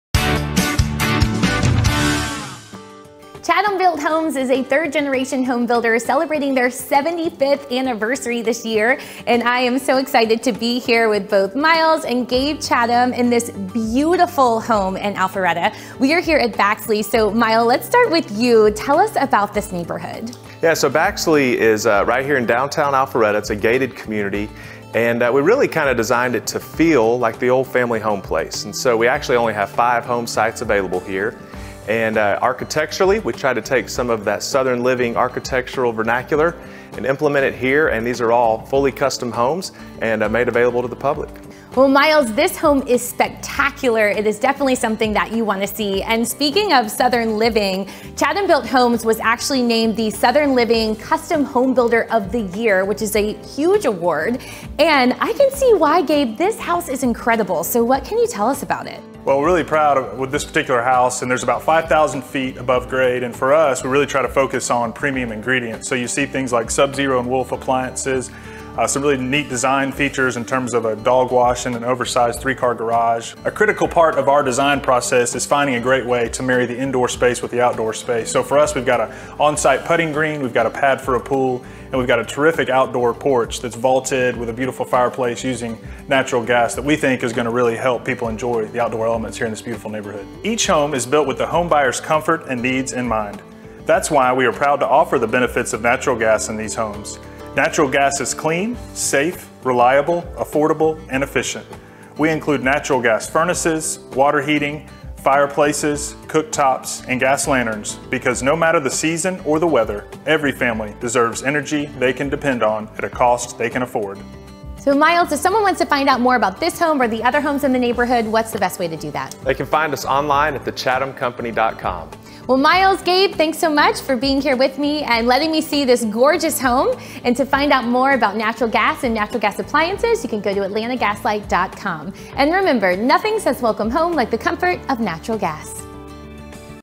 Baxley - Atlanta Gas Light / The Chatham Company 2230
Baxley - Atlanta Gas Light / The Chatham Company
Alpharetta

As Alpharetta’s newest luxury community, Baxley is located off Hopewell Road in Alpharetta’s historic downtown district. The architectural theming within Baxley is inspired by the comforts of the south with the convenience of new urbanism.